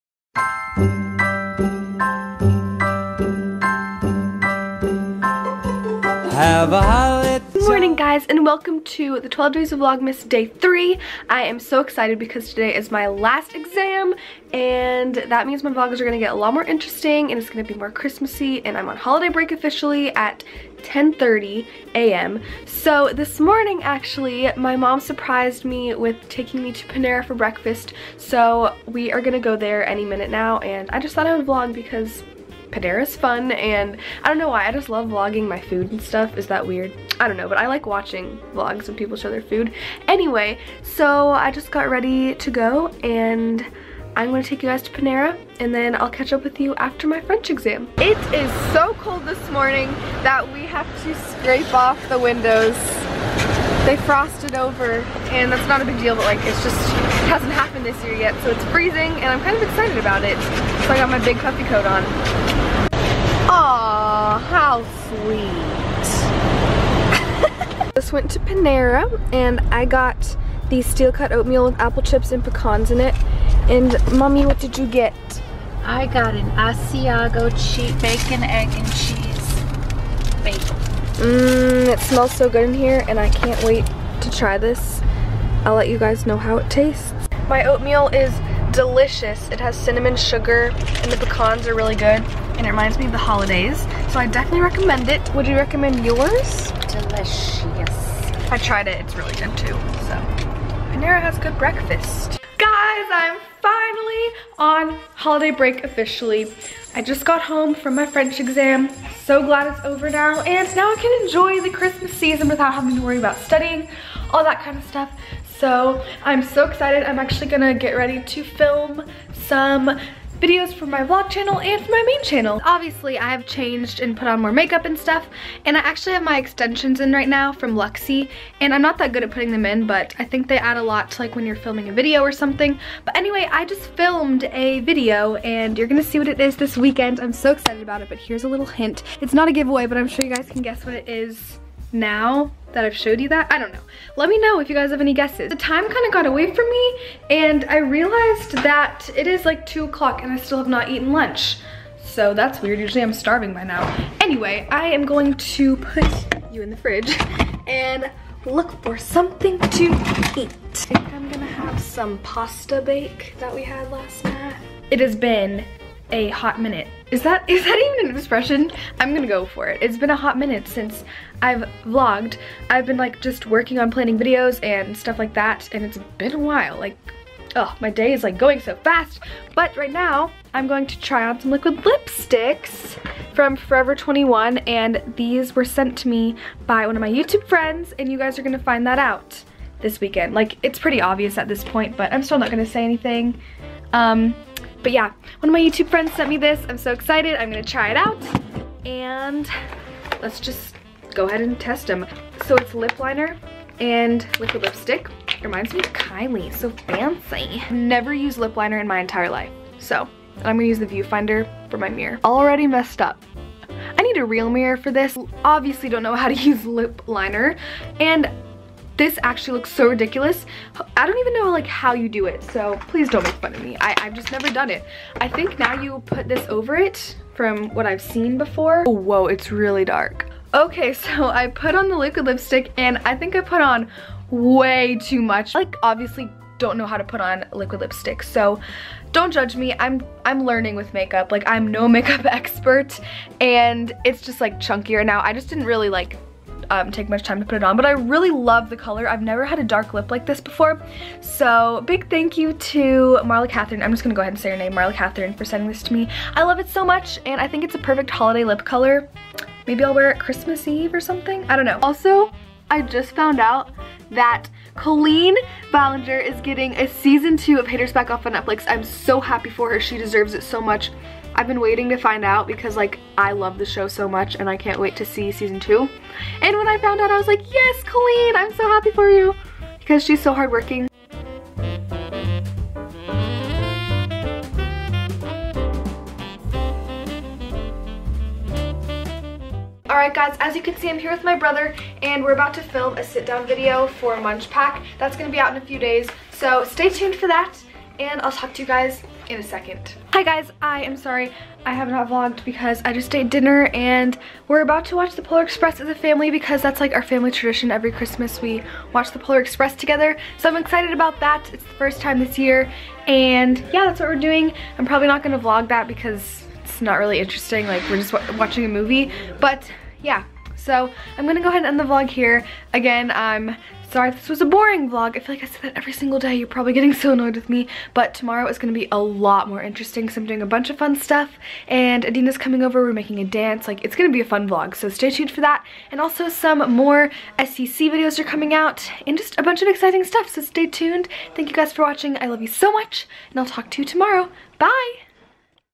Makeup Disaster, Breakfast at Panera, and NO MORE EXAMS!! | 12 Days of Vlogmas {Day 3}
Woohoo for another vloggggg!! Thank you guys for watching 💗